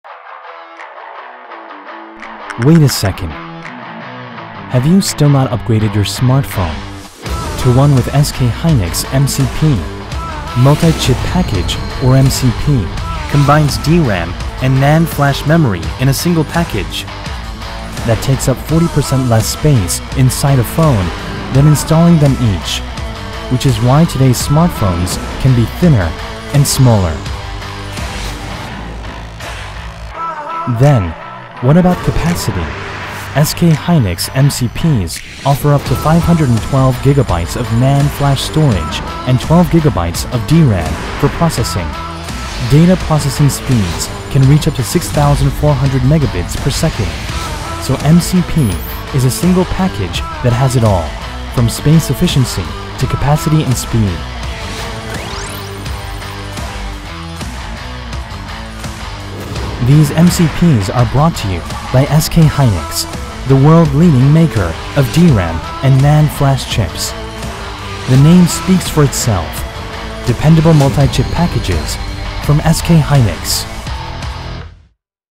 MCPs have upgraded your smartphone.
The Multi-chip packages by SK hynix, the world-leading maker of DRAM and NAND flash chips. The name speaks for itself!